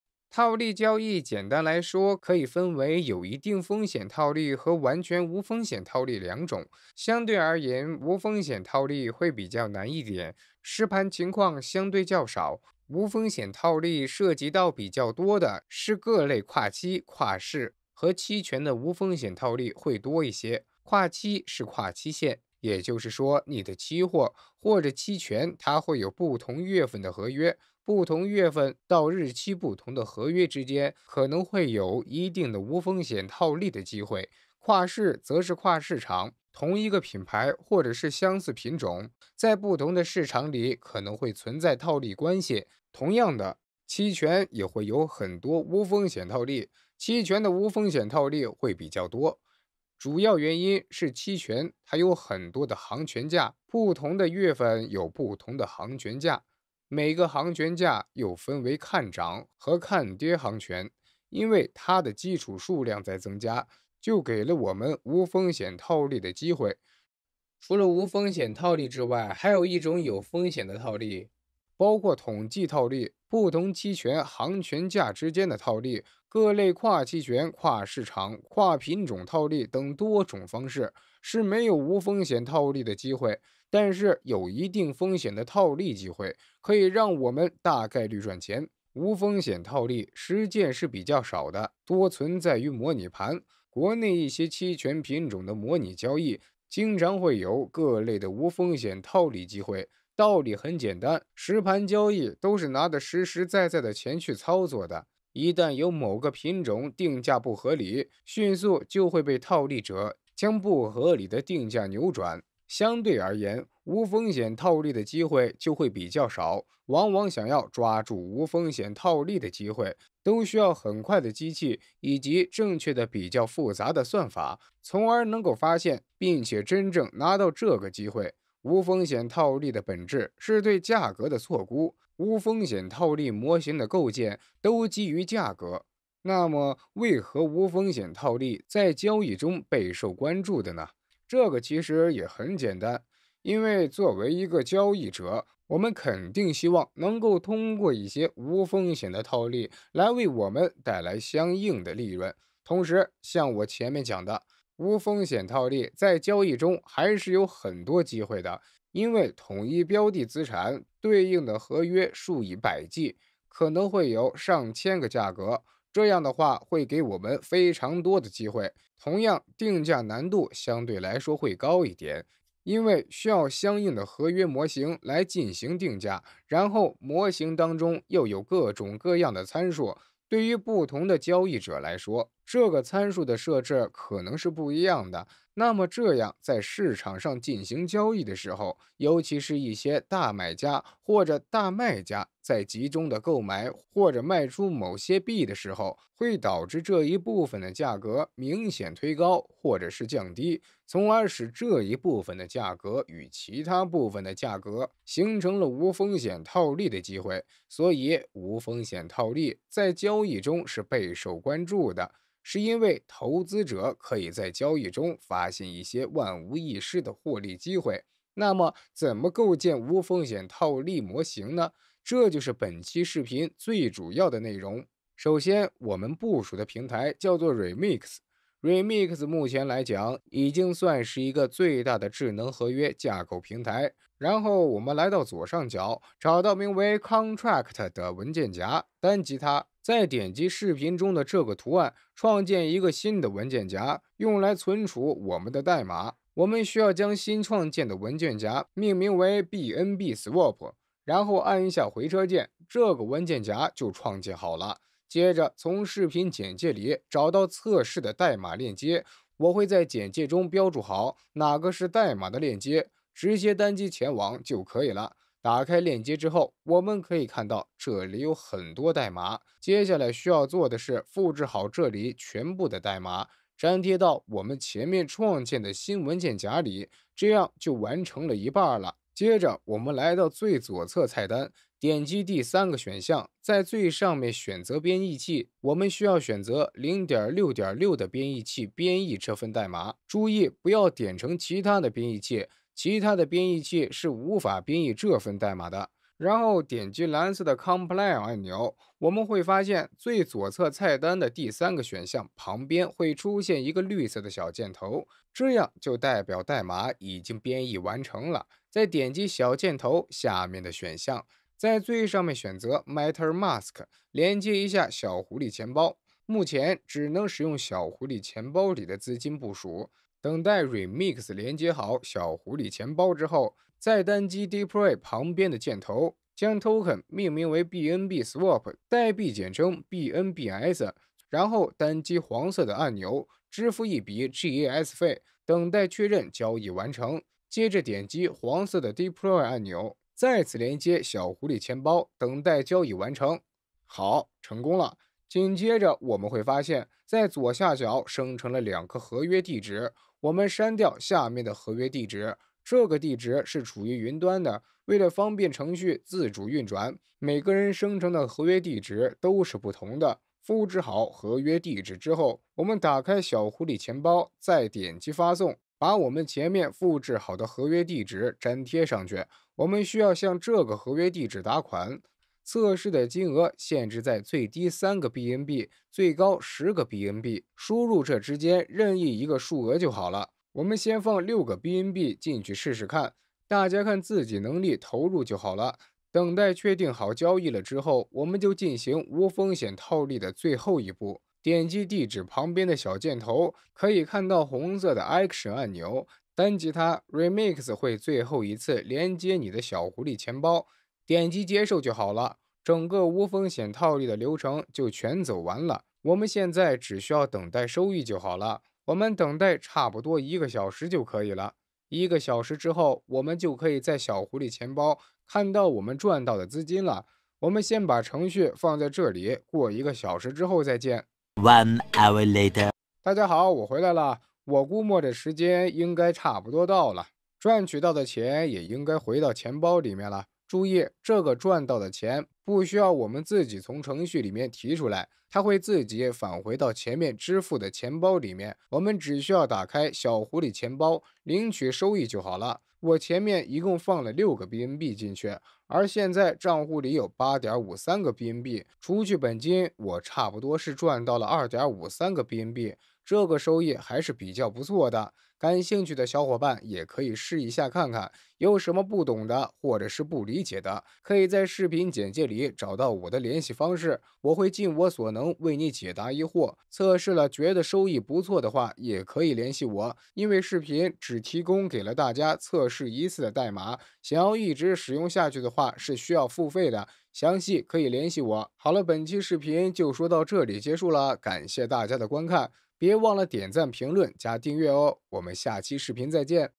如何通过 Pancakeswap 每天赚取 1000 美元 | 无风险套利教程 (Binance bot | 量化交易) 無需下載
本视频将展示无风险套利模型的搭建方法，模型可以自动检索万无一失的获利机会，并及时做出行动抓住这些机会，实现获利

** 更新：
1. 我们需要连接2次小狐狸钱包，再去投入资金，如果第一次连接没有跳转到小狐狸钱包页面，可以手动点击一下小狐狸钱包的标识连接
2.测试的金额限制在最低3个最高10个BNB，建议投入5-10个BNB收益更高，大家看自己能力投入，如果没注意资金限额，可以参考下方的描述将自己退回
3.操作过程中我们会创建出两个合约地址，我们需要删除下面的地址，然后向上面的地址转入测试资金

** 常见问题[非常重要，含合约资金退回方法]
Q1: 如何退回存放在合约中的BNB？
A1: 在之前部署的合约中转入0.0001bnb[程序设定的退币函数值]+转入的资金BNB，然后执行action。例如：在部署合约后转入3bnb，在需要退回资金时，需要再向合约转入3.0001bnb，然后action，合约将会执行退币函数，退回之前的3bnb一共6.0001bnb到你的钱包里

Q2: 如何继续存入BNB？
A2: 存入任何大于3的BNB即可。合约添加了保护机制，为了资金命中率，如果单次少于3 BNB ，合约将暂停执行，直到达到所需金额，不过你如果想要退回资金，可以参照上一个问题的描述

Q3: 合约运行后没有收益产生怎么办？
A3: 可以参考上一问题，主要原因是存入资金较少，合约为了保证命中率，会暂时拒绝交易，直到达到所需金额，你可以选择参考Q1退出资金或者添加至少3 BNB

无风险套利在交易中还是有很多机会的，因为同一标的资产对应的合约数以百计，可能会有上千个价格，这样的话会给我们非常多的机会，同样定价难度相对来说会高一点，因为需要相应的合约模型来进行定价，然后模型当中又有各种各样的参数，对于不同的交易者来说，这个参数的设置可能是不一样的，那么这样在市场上进行交易的时候，尤其是有一些大买家或者是大卖家，在集中的购买或者卖出某些币的时候，会导致这一部分的价格明显推高或者是降低，从而使这一部分的价格与其他部分的价格形成了无风险套利的机会

** 部署流程：
第 1 步 - 在 Firefox、Edge 或 Chrome 中设置 MetaMask 钱包
第 2 步 - 将币安智能链网络添加到 MetaMask
第 3 步 - 转账BNB到您想要使用无风险套利模型的钱包中
第 4 步 - 转到 Remix 官网（用于编写智能合约）
第 5 步 - 单击合约文件夹，然后单击“创建新文件”按钮
第 6 步 - 将文件命名为 BNBSwap
第 7 步 - 从下面的链接中复制并粘贴合约代码链接到网页打开
第 8 步- 单击左起第二个选项卡
第 9 步 - 选择与代码顶部 0.6.6 相匹配的编译器
第 10 步 - 单击编译
第 11 步 - 转到左侧的第三个选项卡
第 12 步 -对于环境，将其更改为“Metamask”，然后在小狐狸钱包中确认已连接
第 13 步 - 单击部署旁边的箭头并输入您全新令牌的信息，然后单击交易
第 14 步 - 部署合约后，点击复制合约代码并前往 MetaMask 存入流动性
第 15 步 - 完成后，点击你的新合约并点击红色按钮（action/execute/flashloan）

** 补充:
注意：本教学视频仅针对BSC链，与以太坊链不适配，后期会单独开一期视频讲解